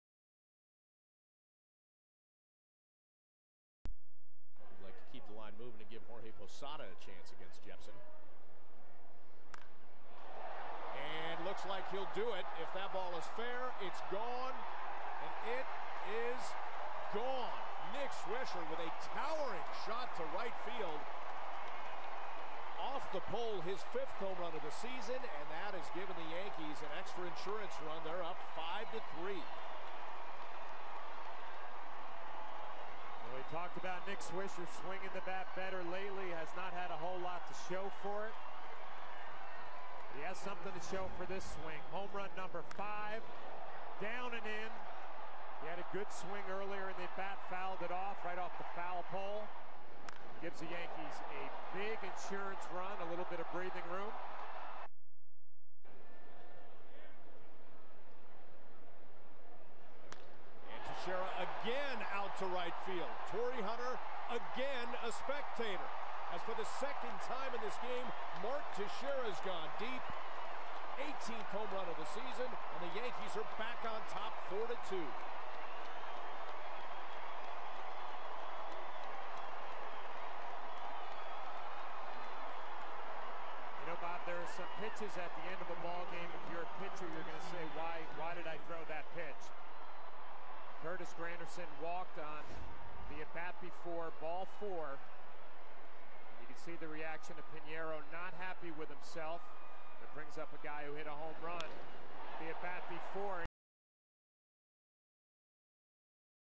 Yankee Highlights of Last Night
The best highlights of the Yankees v.s. Angels